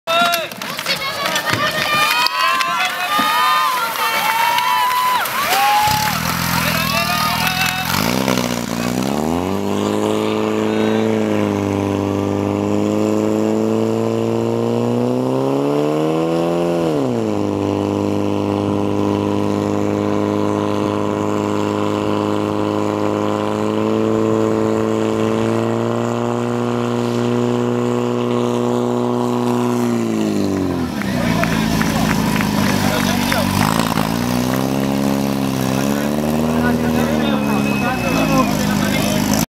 Hasiči Hůrka - Požární útok na 3B muži Hůrka v Janovicích (31,14 s)
Akce: Okrsková hasičská soutěž muži, ženy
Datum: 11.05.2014
Místo: Janovice
2x požární útok, pro nepřízeň počasí bez běhu jednotlivců 100 m překážky